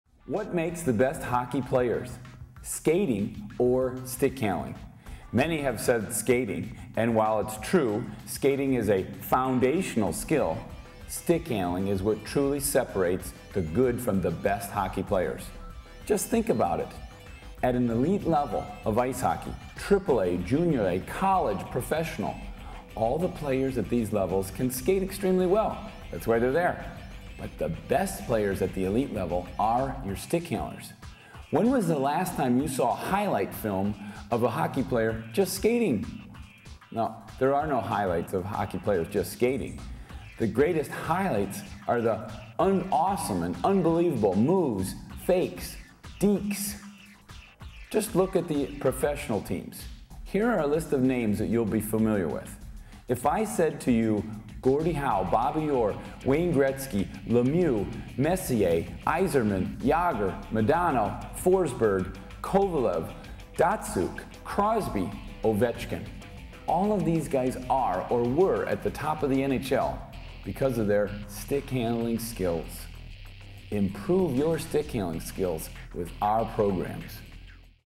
What makes the best hockey players?
Online Stickhandling is a new website created out of a partnership between Lance Pitlick of Sweet hockey and Sean Skinner of Skinner Hockey. Here, Sean discusses what makes the best hockey players.